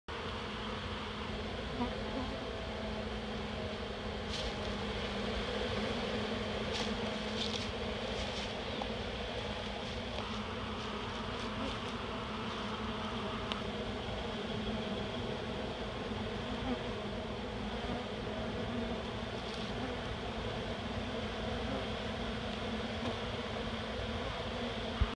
Capture of Bees in the Tree from Greenville, AL
You can hear the buzzing of the bees. I must have been lucky enough to capture the queen because the bees kept on going in following her pheromones.